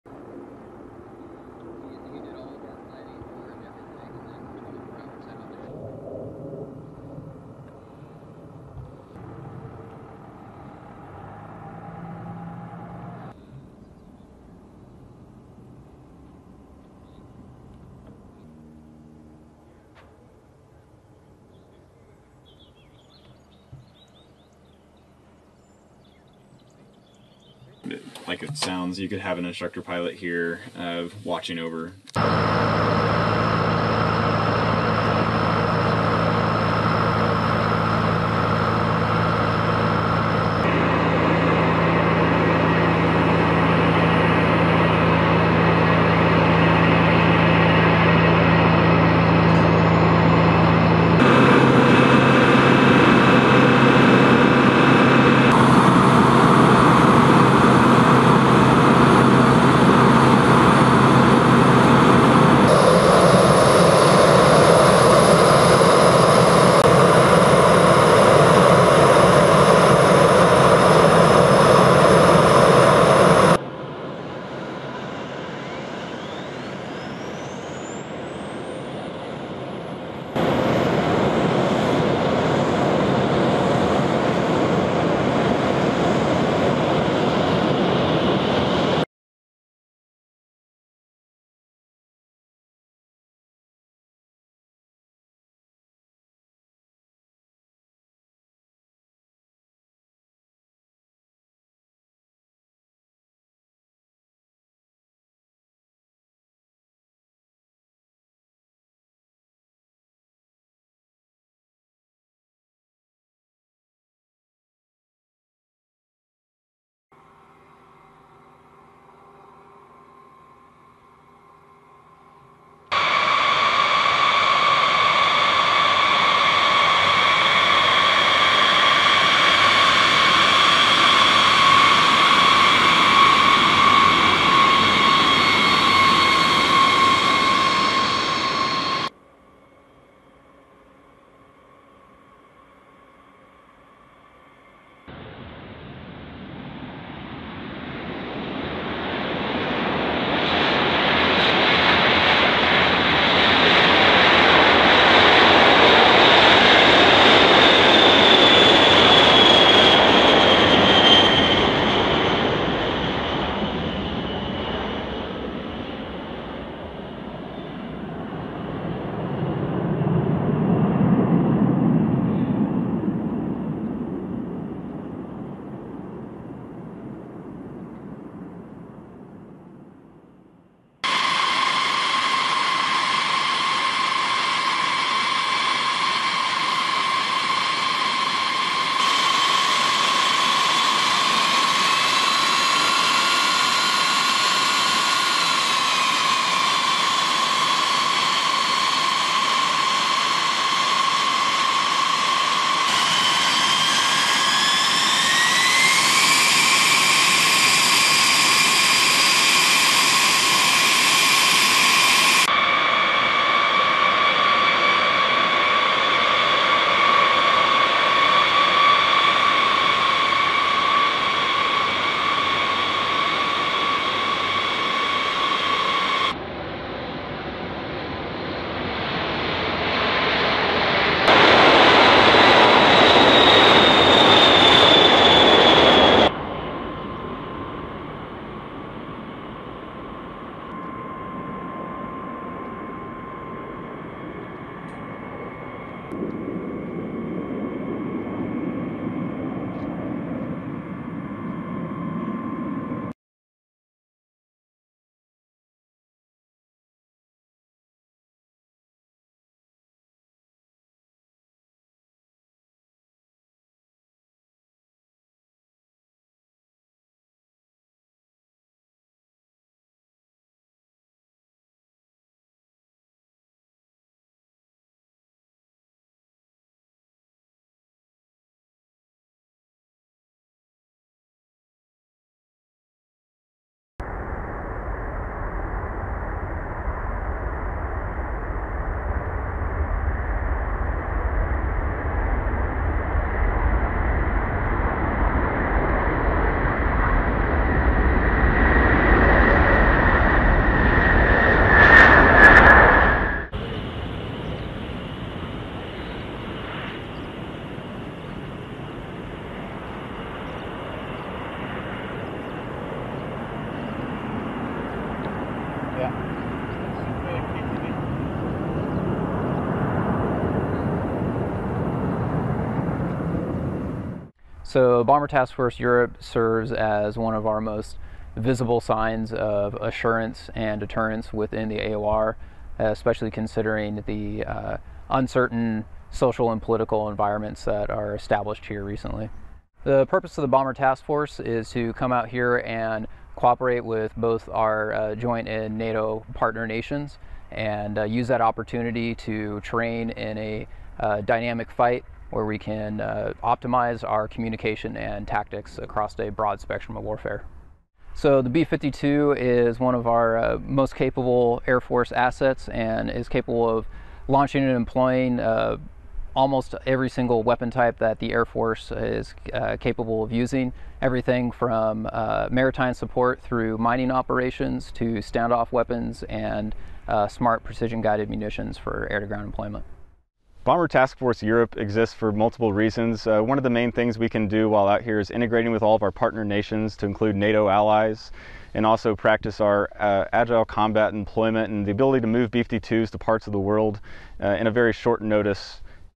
Despliegue de los bombarderos B-52 en Europa
Los bombarderos B-52H Stratofortress de la Fuerza Aérea de EE. UU. concluyeron recientemente un despliegue de un mes en el Reino Unido como parte de Bomber Task Force Europe.

Defensa.Com